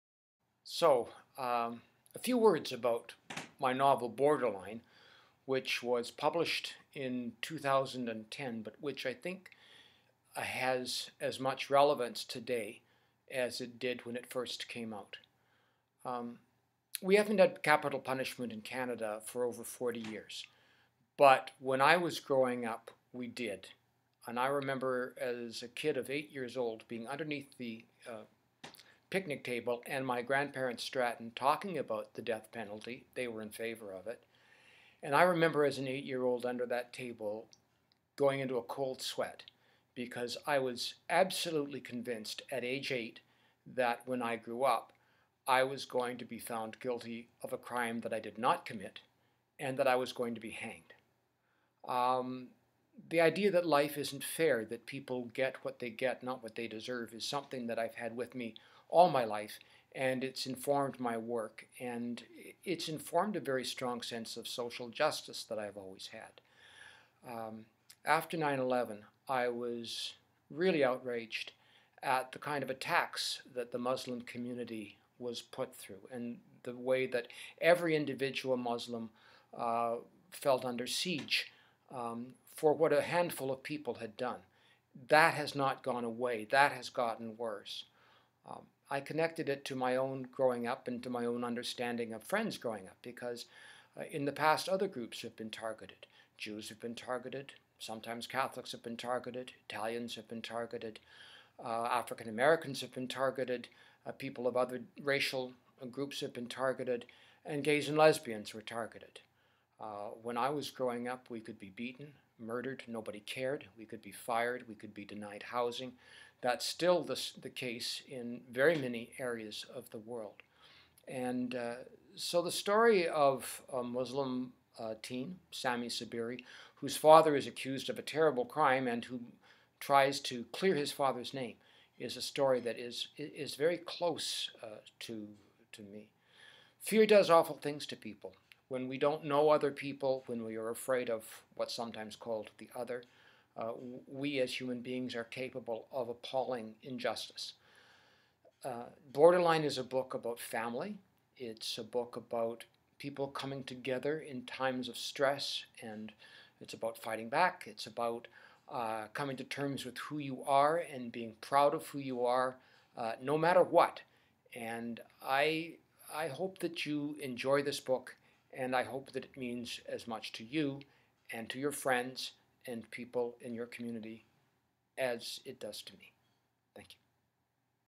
About Borderline
Allan talks about discrimination, fear, and the background of his ALA Best Book BORDERLINE.